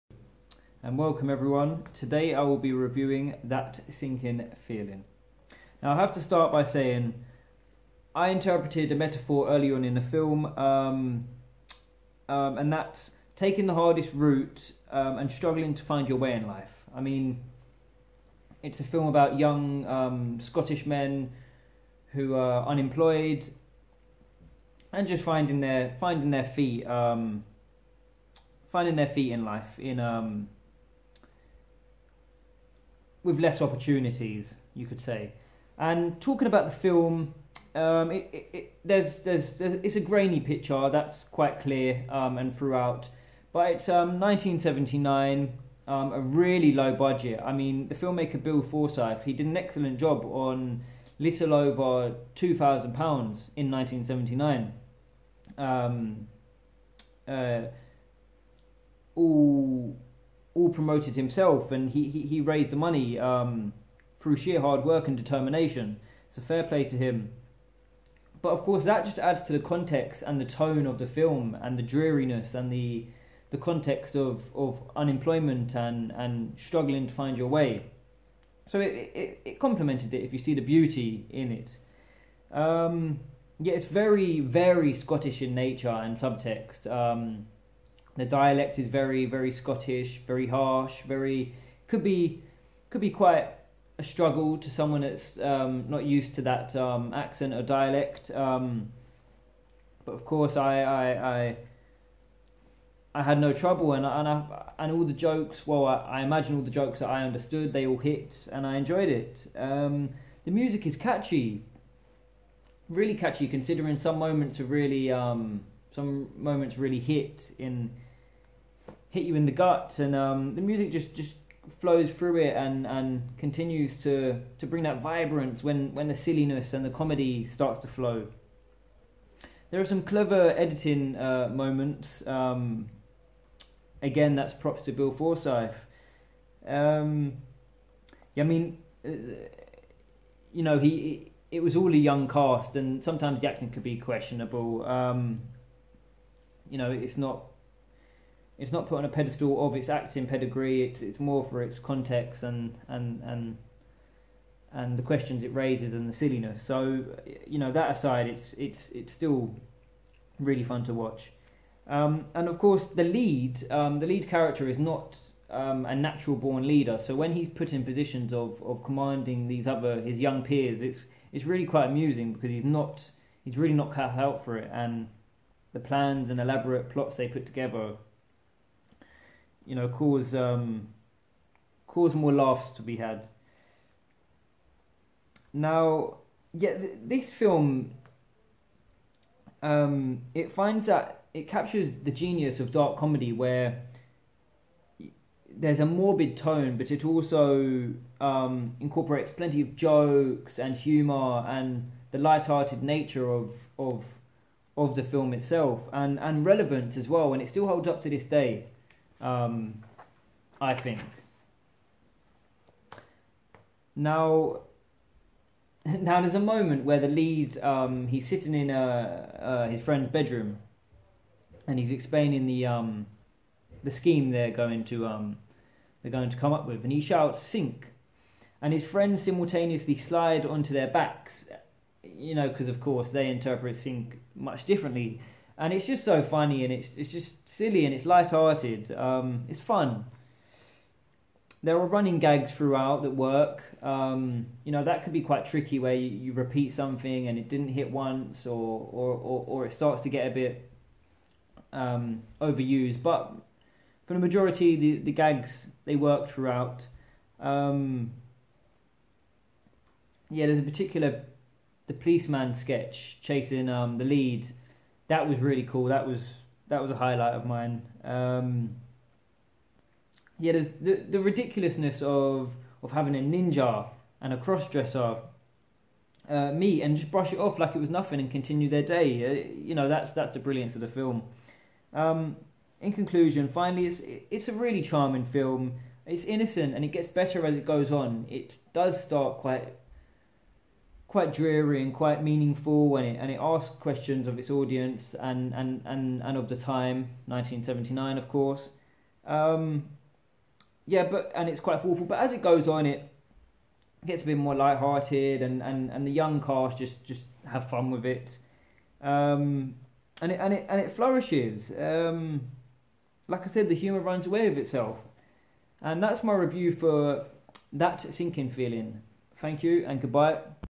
That Sinking Feeling 1979 : movie review
today I am reviewing:

That Sinking Feeling ( 1979 )

Directed by :
Bill Forsyth

* all thoughts and opinions are soley my own